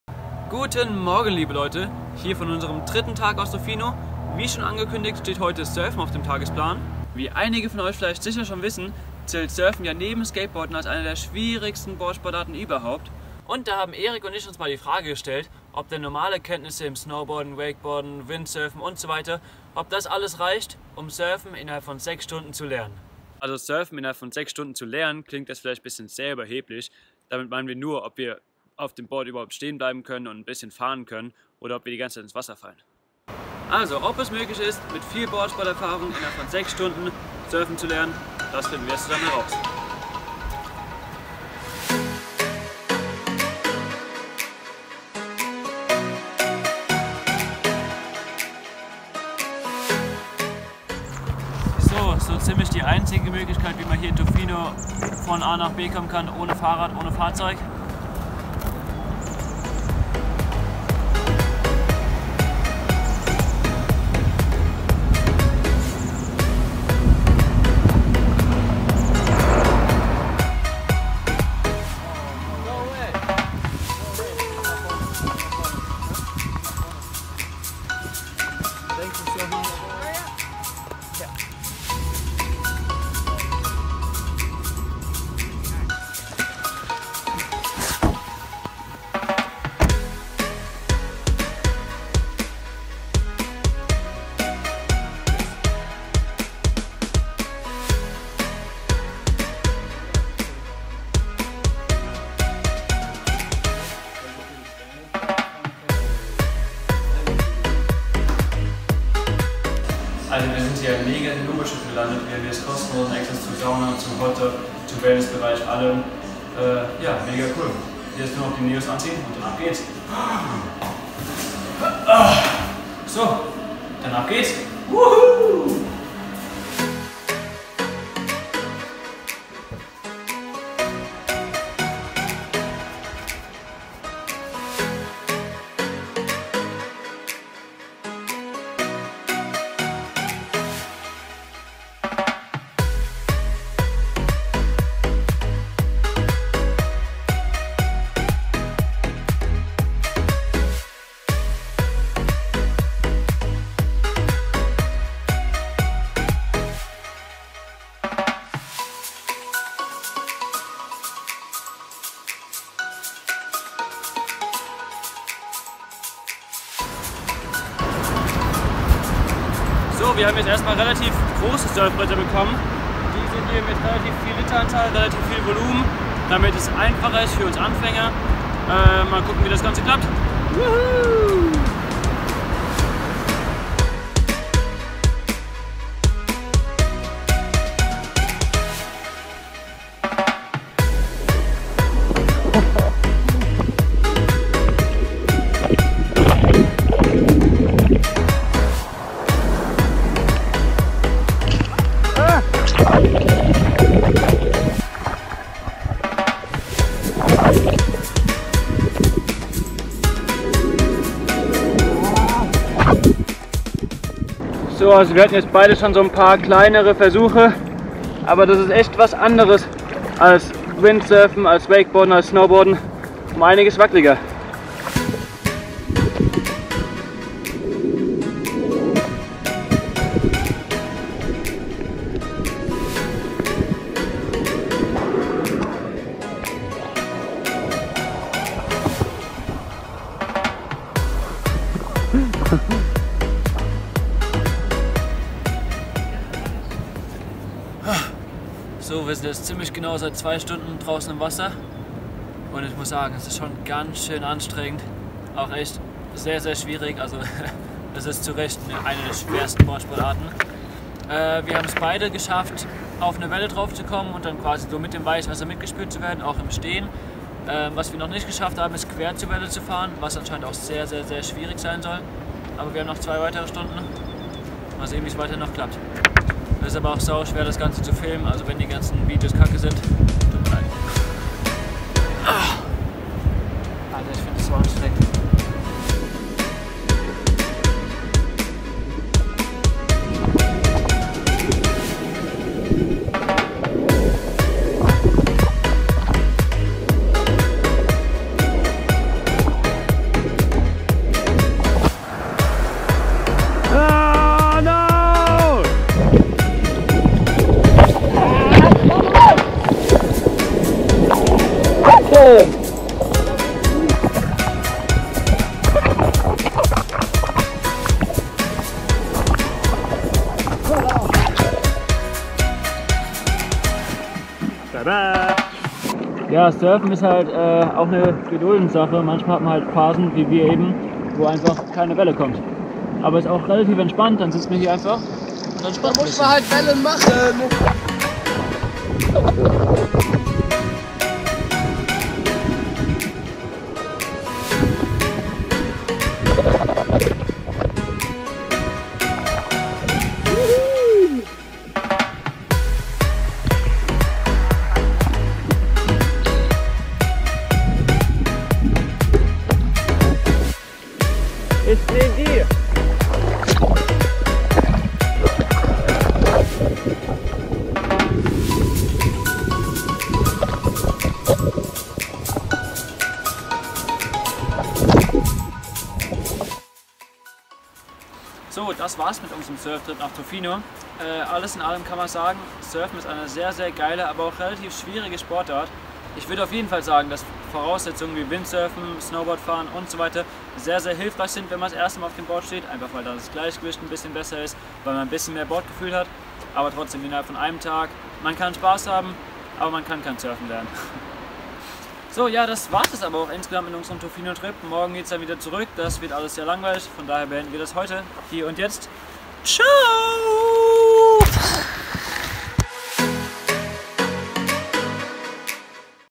Lean how to surf in 6 hours? | KANADA VLOG #20
Of course we had to try surfing in canada`s Surf-capital, Tofino.

How our first time surfing worked out, weather we just kept falling or actually were able to surf a bit, thats what you`ll find out in the video! :)